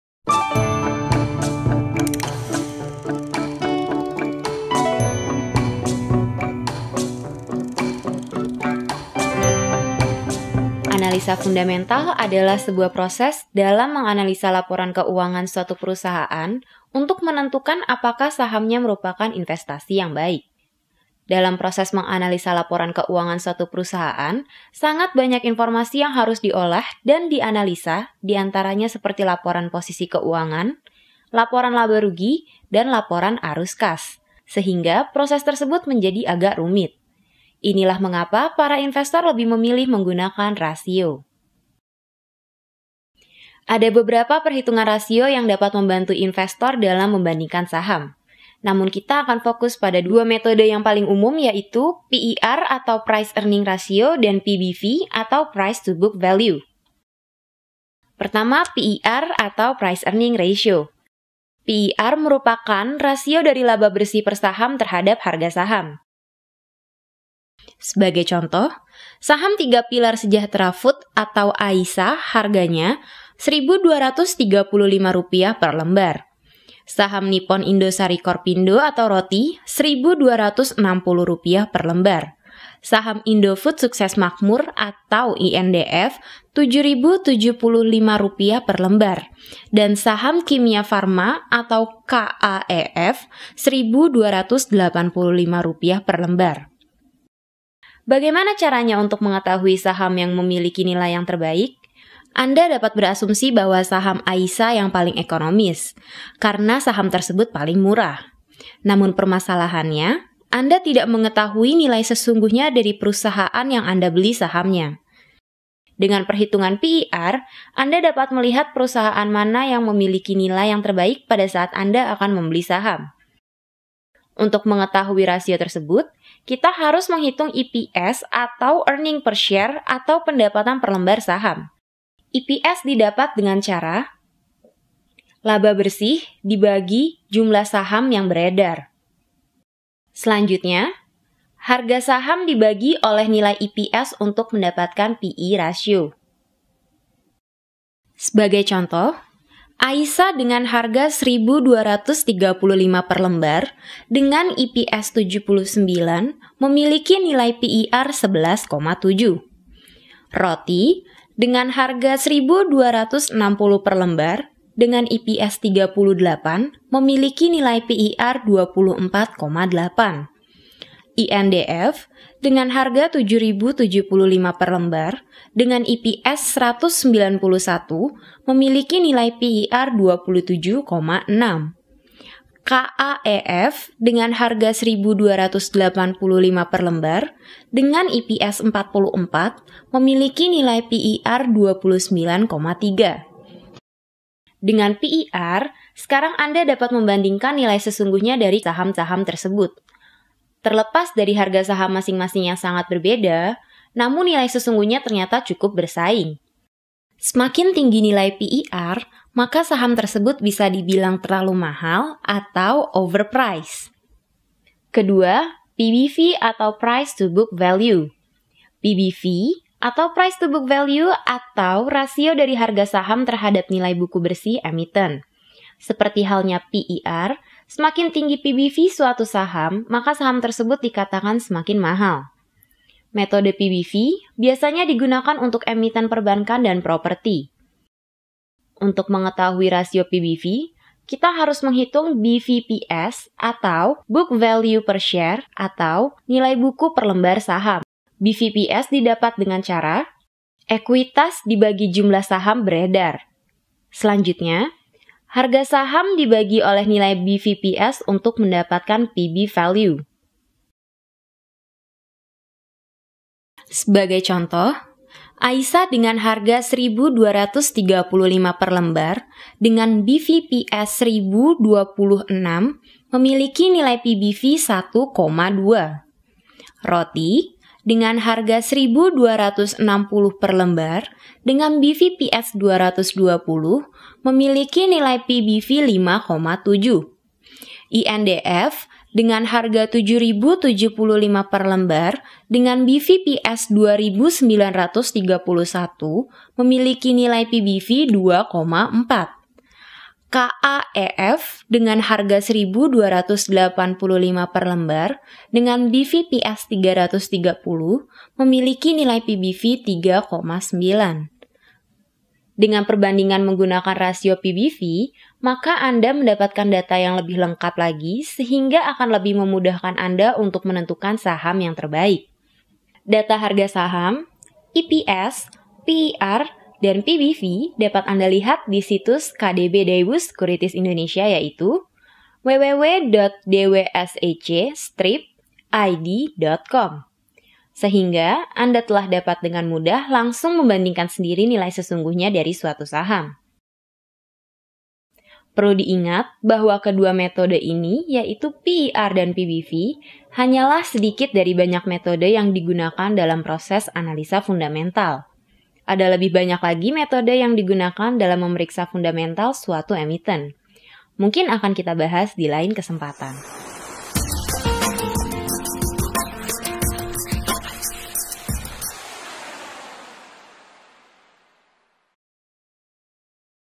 Untung 100$/Hari "Analisa Fundamental"
Dengan mengananalisa Saham kita dapat mengetahui bagaimana cara mengetahui kapan harus membeli dan menjual.